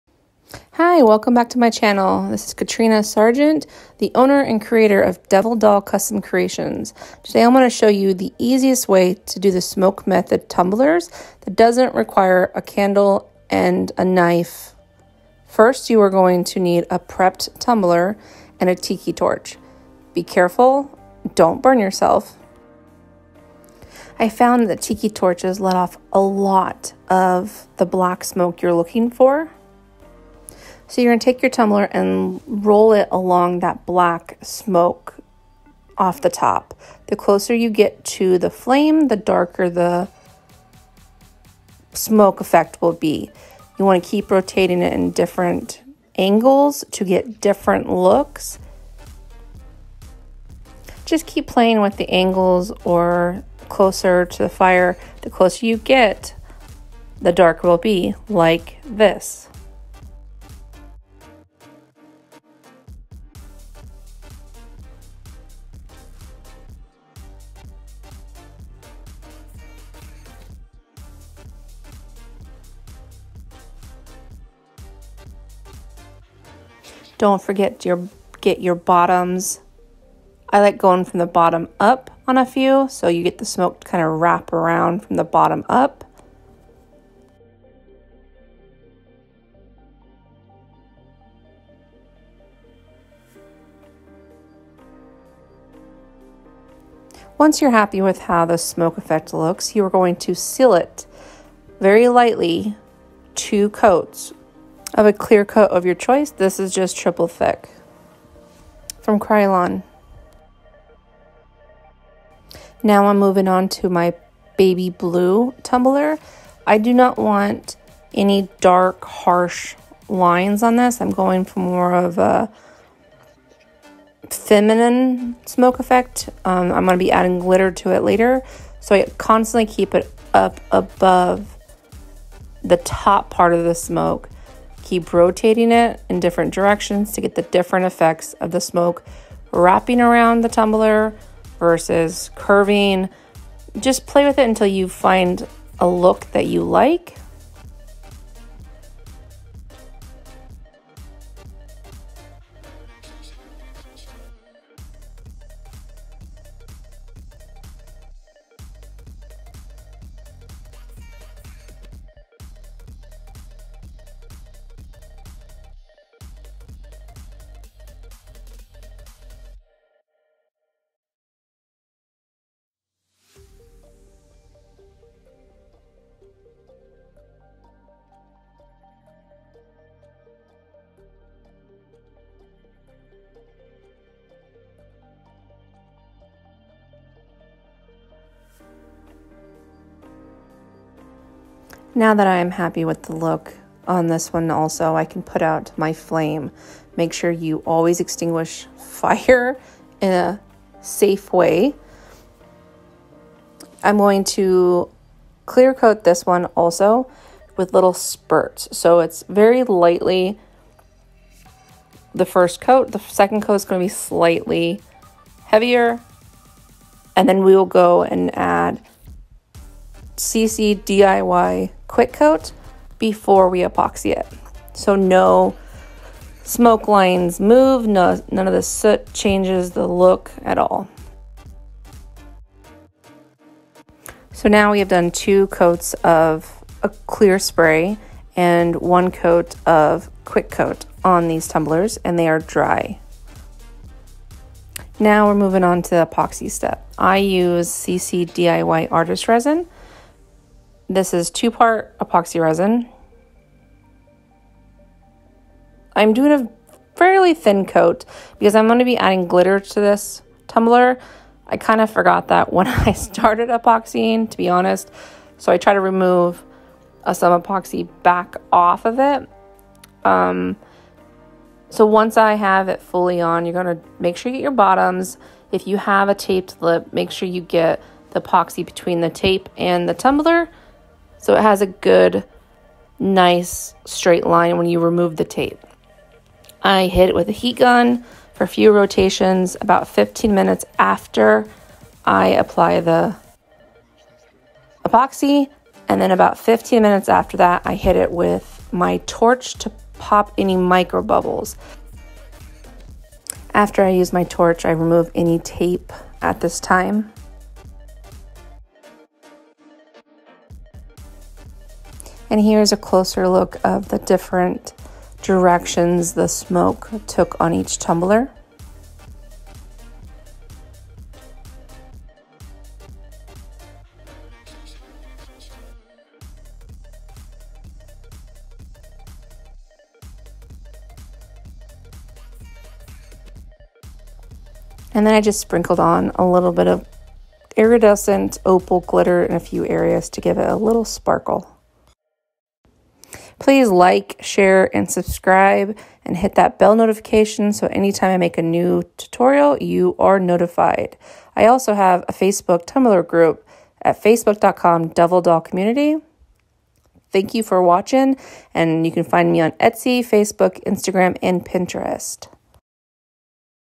The easiest smoke method tumbler tutorial
The easiest smoke method you will ever find!

Rust-o-oleum Crystal Clear enamel -or- Rust-o-oleum 2x ultra clear -or- Krylon triple thick clear
I use all three, you can find them at any hardware store

Tumblers:
white smoke tumbler 16oz Contigo coffee mug
baby blue tumbler is a 3 in 1 REDUCE COLD-1 CAN/BOTTLE COOLER from stainless steel depot

Glitters
sass and crafts llc
Golden Opal
Opal Glitz fine blend